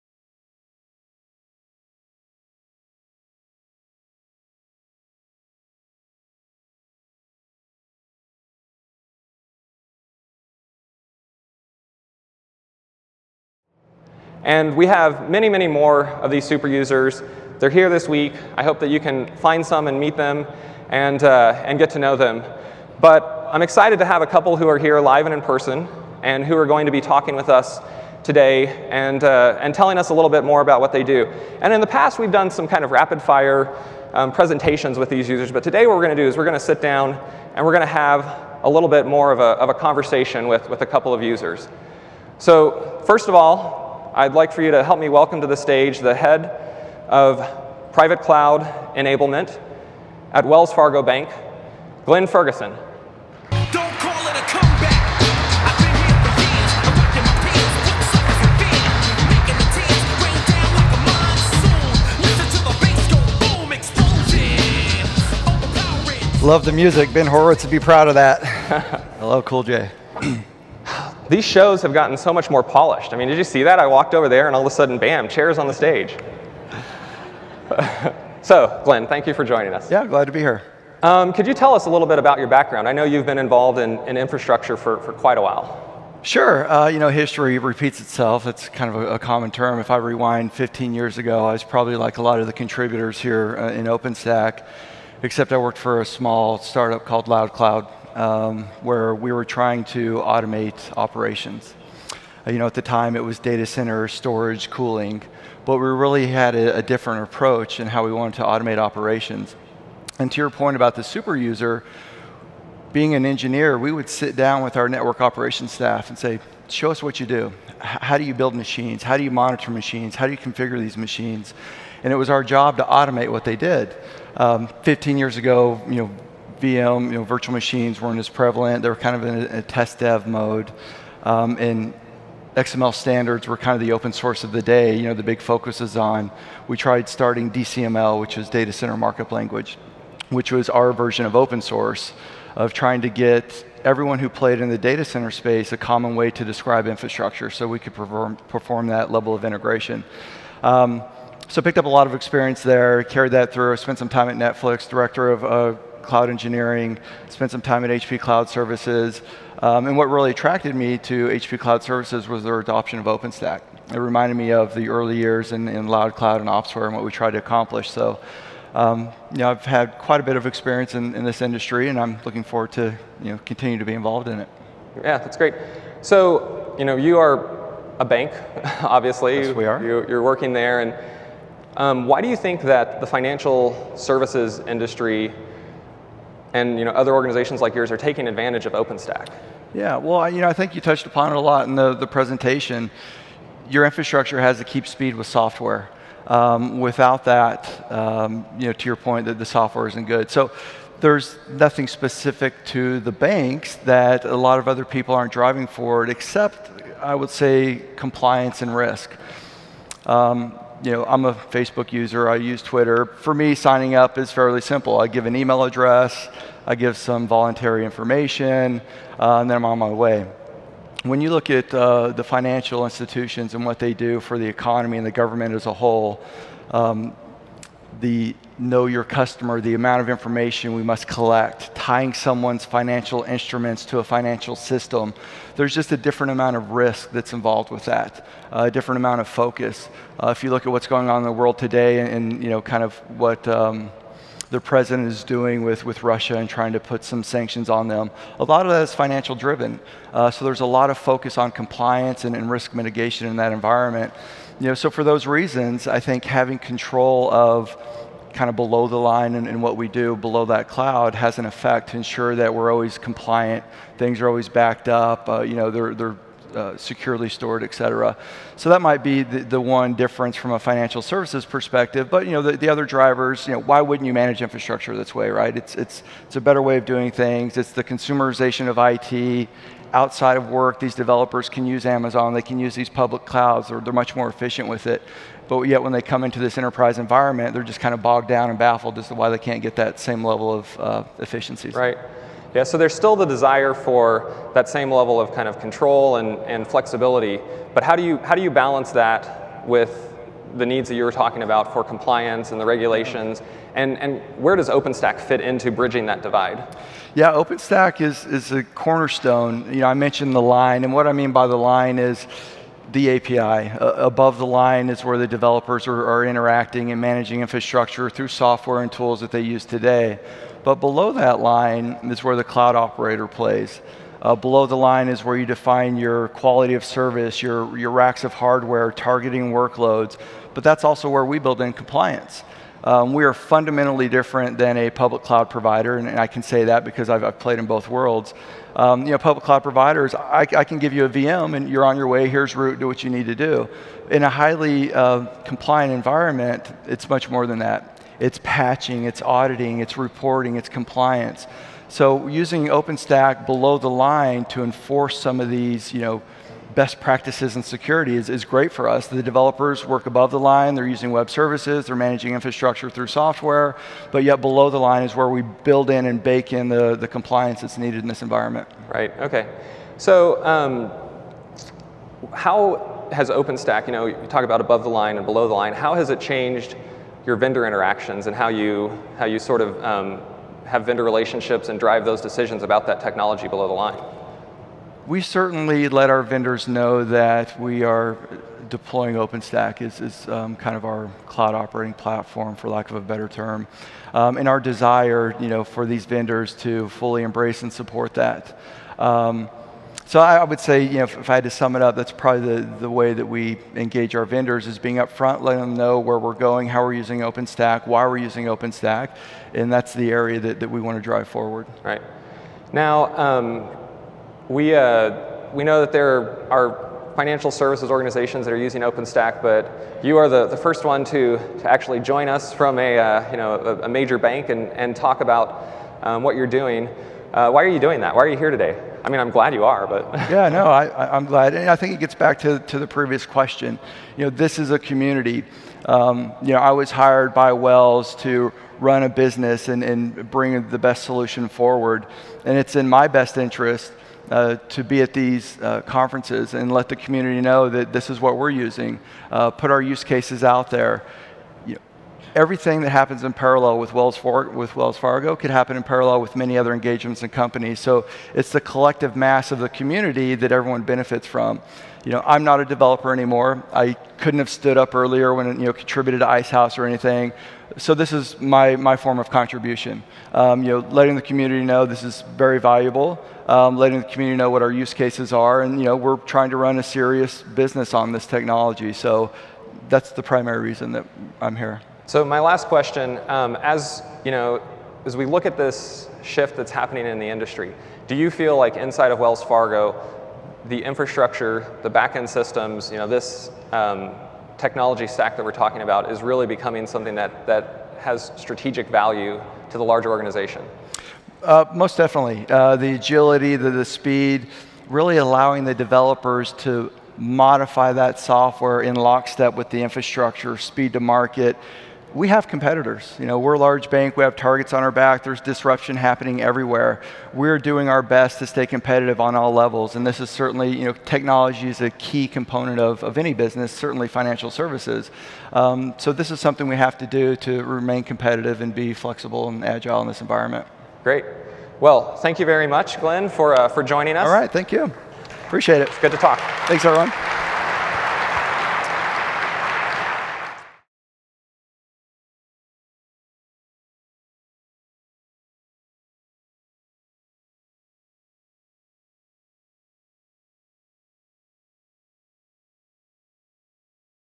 Atlanta 2014 Keynotes: OpenStack User Case Study: Wells Fargo
Glenn Ferguson of Wells Fargo joins Jonathan to discuss Wells Fargo's relationship with OpenStack.